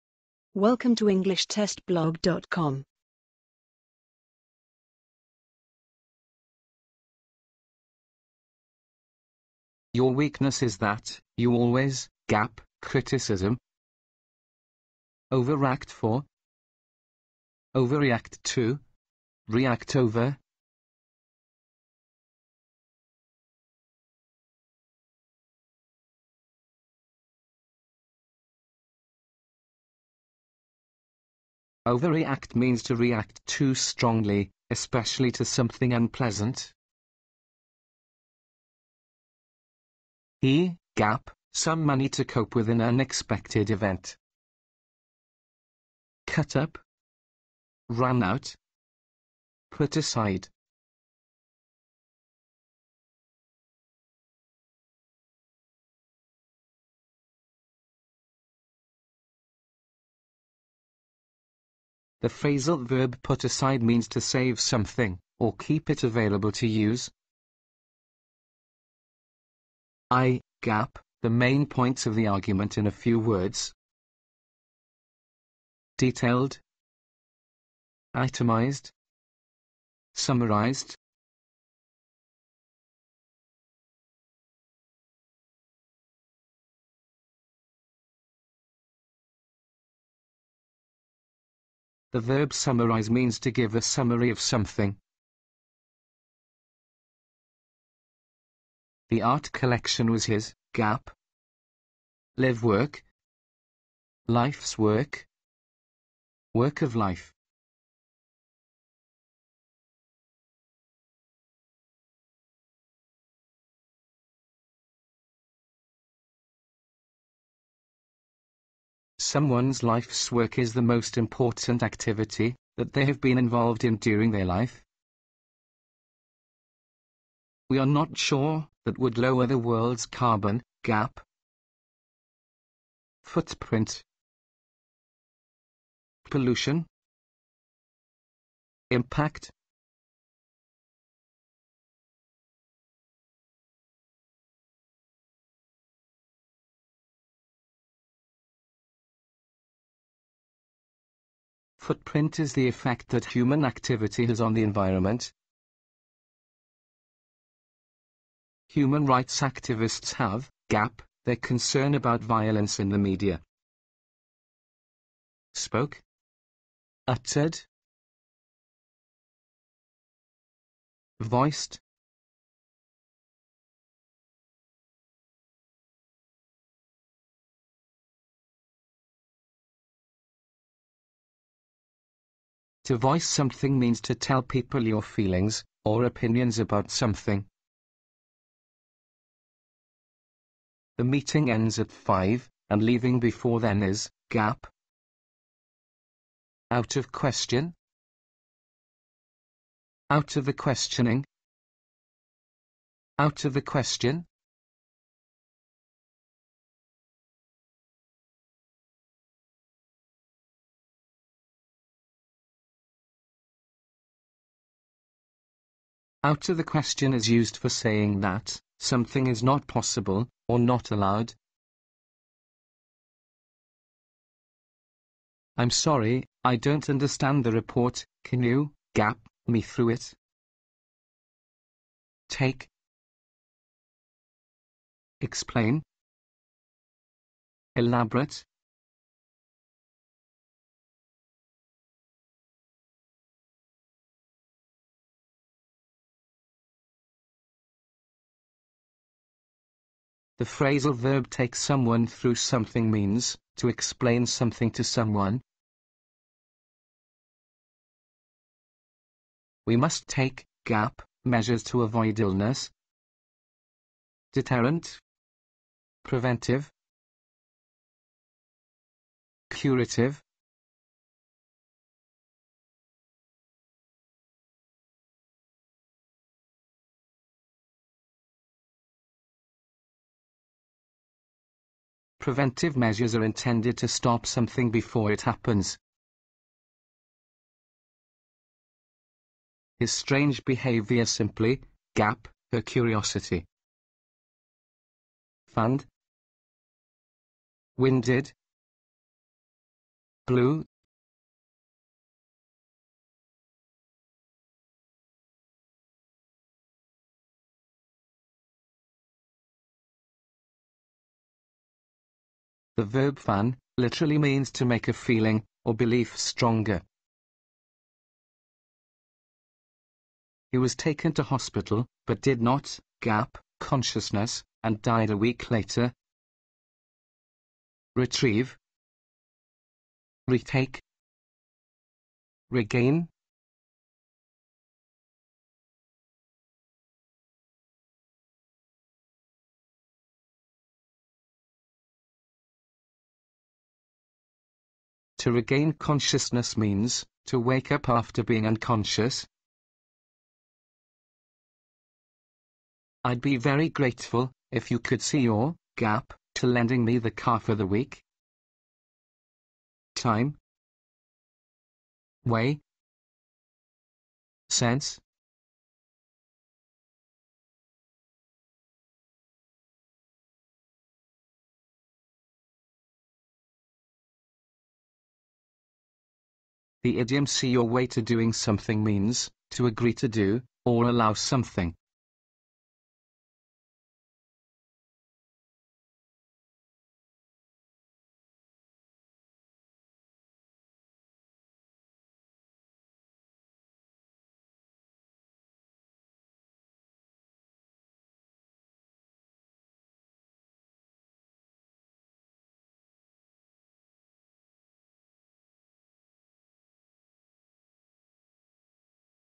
Vocabulary Quiz to Brush Up Your English
There are 12 questions in this quiz.
Brief description: Choose the correct answer to complete the sentence.

QUESTIONS
00:00 - Introduction
00:09 - Your weakness is that you always (overact for/overreact to/react over) criticism.
00:40 - He (cut up/run out/put aside) some money to cope with an unexpected event.
01:09 - I (detailed/itemized/summarized) the main points of the argument in a few words.
01:39 - The art collection was his (live work/life's work/work of life).
02:06 - We are not sure that would lower the world's carbon (footprint/pollution/impact).
02:39 - Human rights activists have (spoke/uttered/voiced) their concern about violence in the media.
03:11 - The meeting ends at 5, and leaving before then is (out of question/out of the questioning/out of the question).
03:45 - I’m sorry, I don’t understand the report. Can you (take/explain/elaborate) me through it?
04:18 - We must take (deterrent/preventive/curative) measures to avoid illness.
04:49 - His strange behaviour simply (fanned/winded/blew) her curiosity.
05:20 - He was taken to hospital, but did not (regain/retake/retrieve) consciousness and died a week later.
05:52 - I'd be very grateful if you could see your (time/way/sense) to lending me the car for the week?